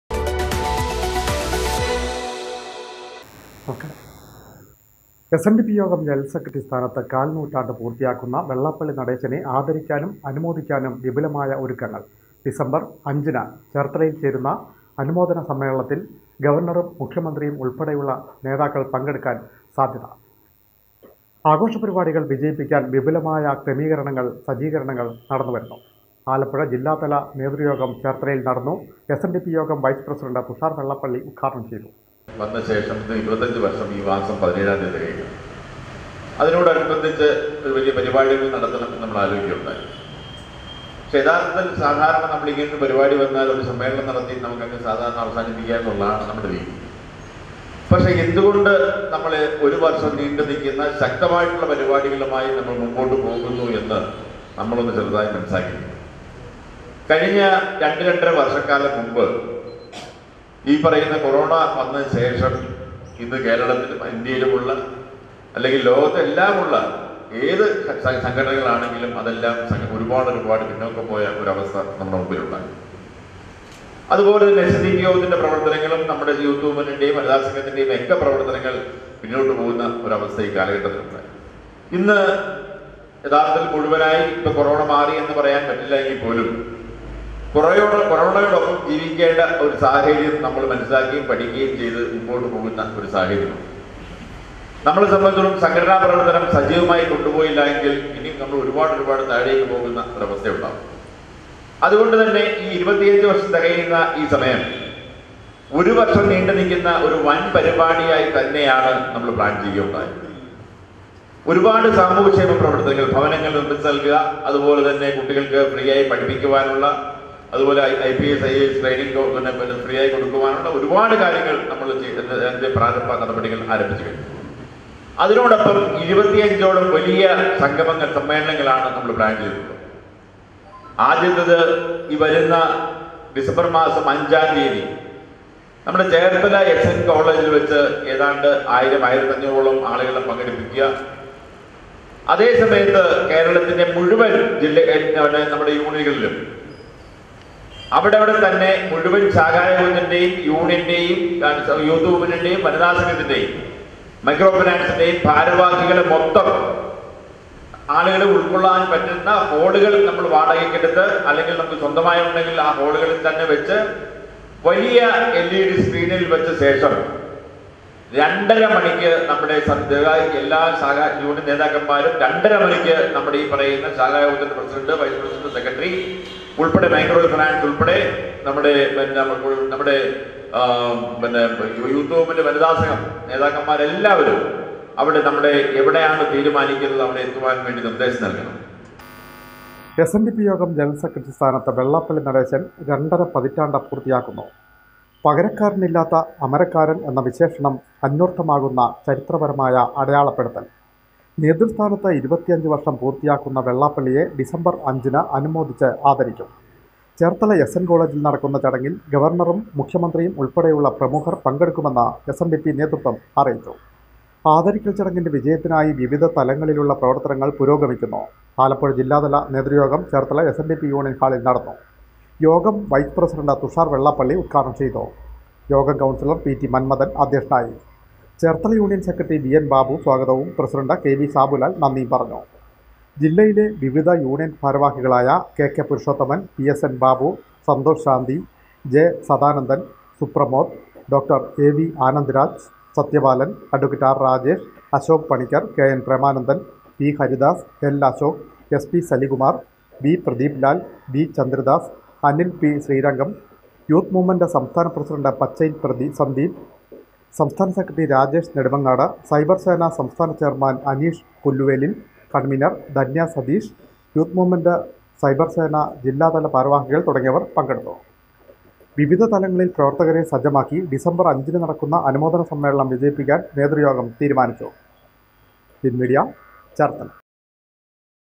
എസ്.എൻ.ഡി.പി. യോഗം ജനറൽ സെക്രട്ടറി സ്ഥാനത്ത് 25 വർഷം പൂർത്തിയാക്കുന്ന വെള്ളാപ്പള്ളിയെ ആദരിക്കുന്നു.
എസ്.എൻ.ഡി.പി. യോഗം ജനറൽ സെക്രട്ടറി സ്ഥാനത്ത്
കാൽ നൂറ്റാണ്ട് തികയ്ക്കുന്ന വെള്ളാപ്പള്ളി നടേശനെ
ആദരിക്കാനും,അനുമോദിക്കാനും വിപുലമായ ഒരുക്കങ്ങൾ.
ഡിസംബർ അഞ്ചിന് നടക്കുന്ന ആഘോഷ പരിപാടികൾ വിജയിപ്പിക്കാൻ
ചേർത്തലയിൽ ചേർന്ന ആലപ്പുഴ ജില്ല നേതൃയോഗം തീരുമാനിച്ചു.